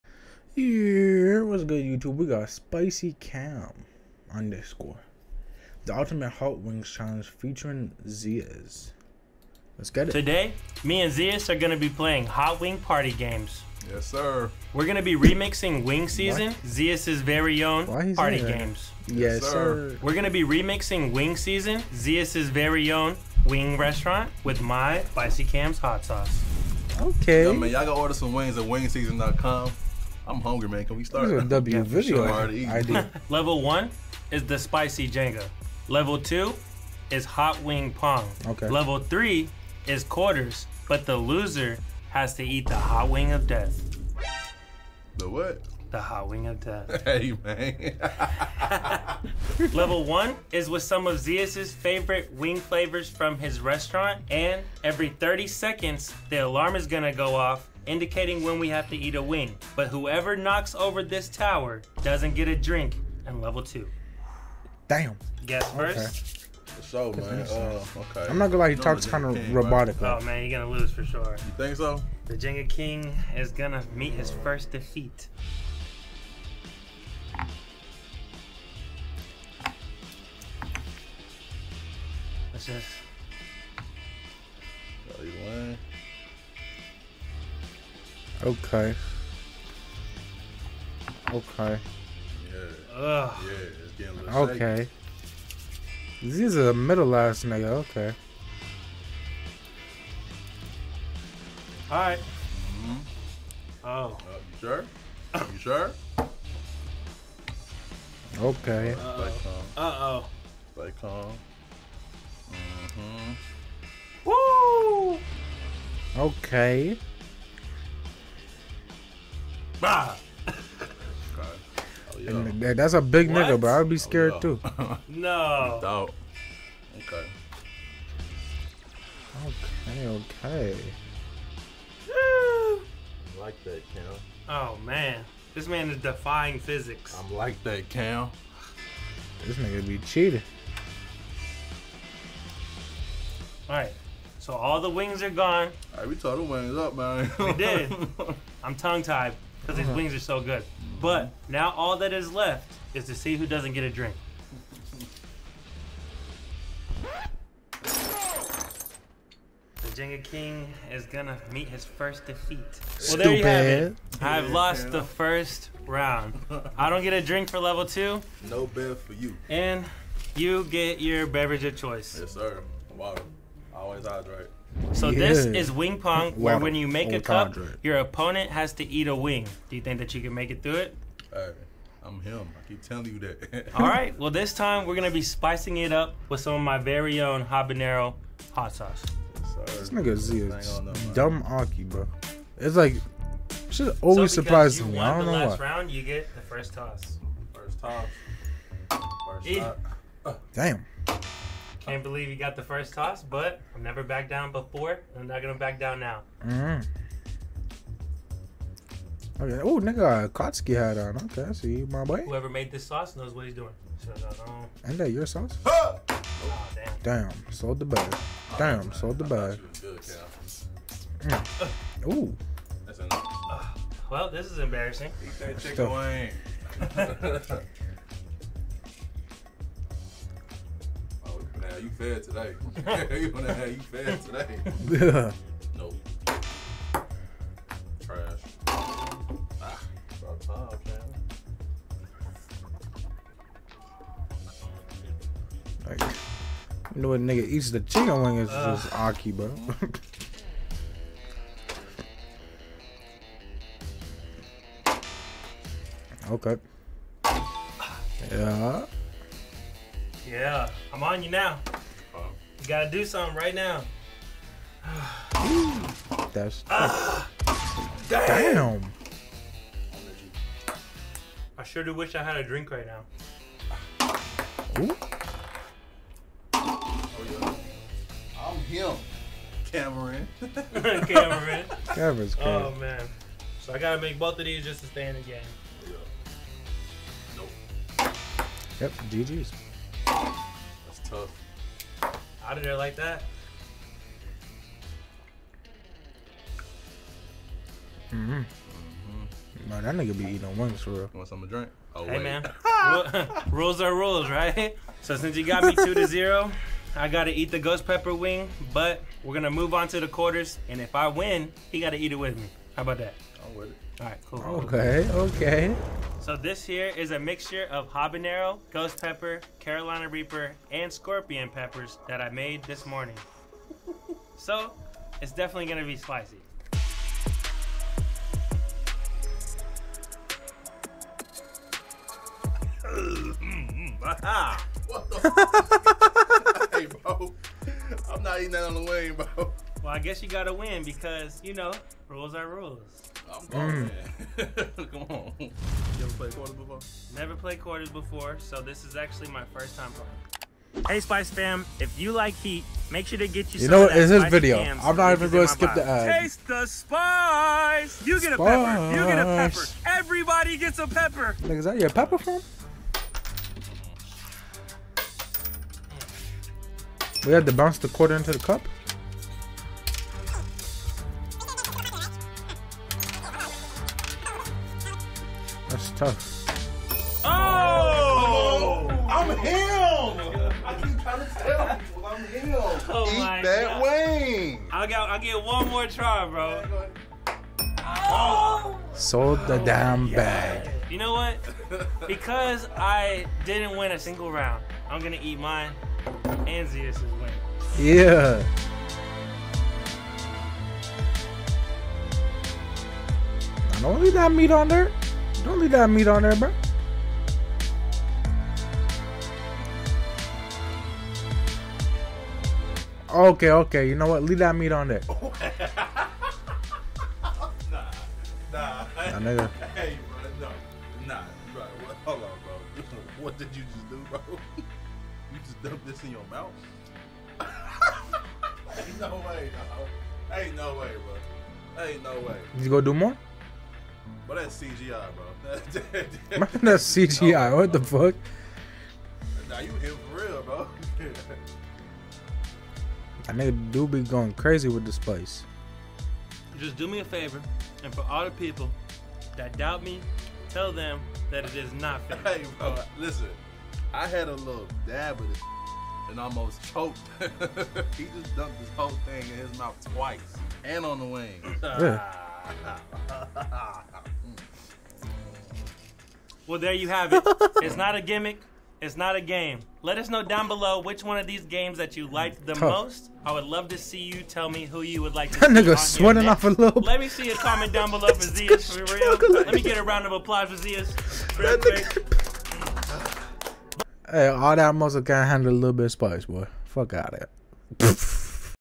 SPICYCAM AND ZIAS EAT HOT WINGS
Main channel: @Trae Soulja